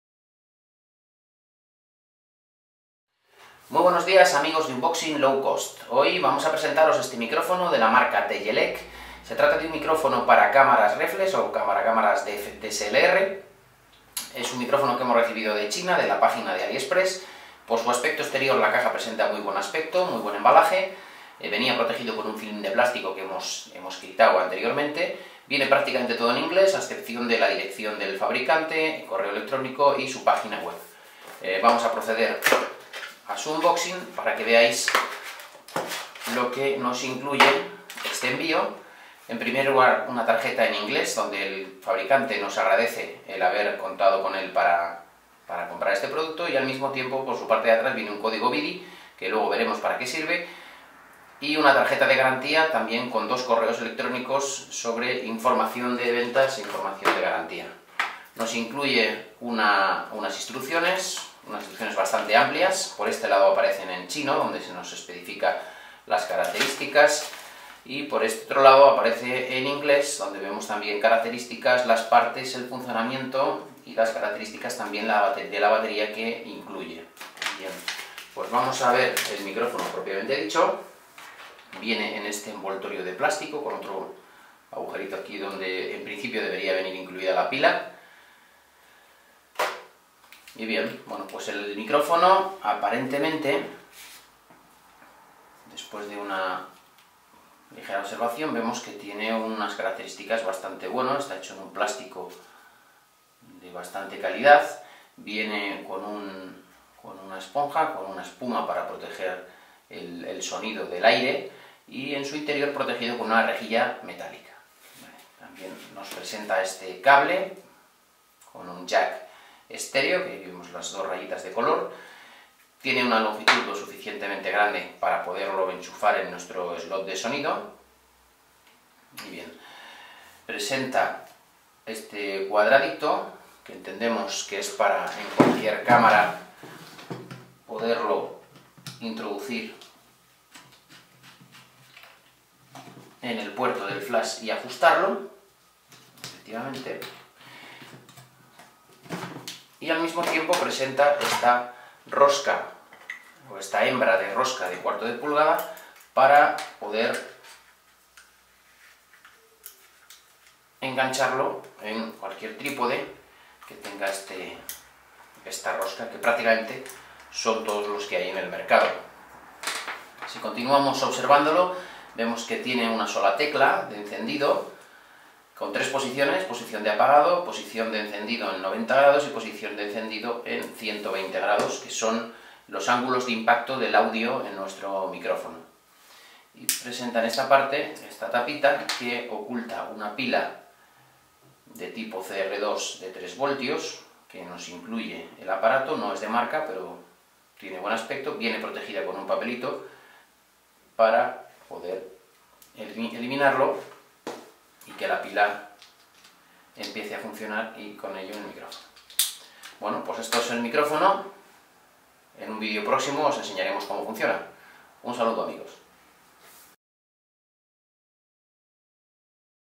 🎤🎙️Micrófono estéreo para cámaras reflex DSLR. U&R Low Cost
Micrófono estéreo para conectar en cámaras de fotos con opción de grabación de audio y video.

Mi Ordenador, iMac
Cámara usada, Nikon d7100